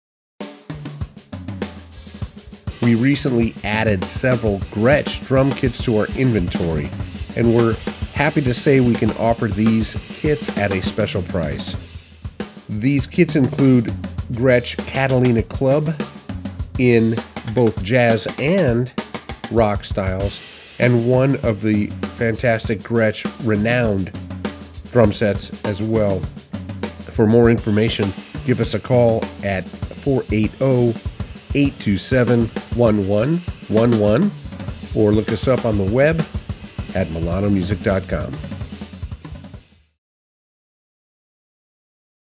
Gretsch Drums in stock at Milano Music Center
We have special pricing on some Gretsch drum sets in stock.
Milano Music Center is a family owned full service music store.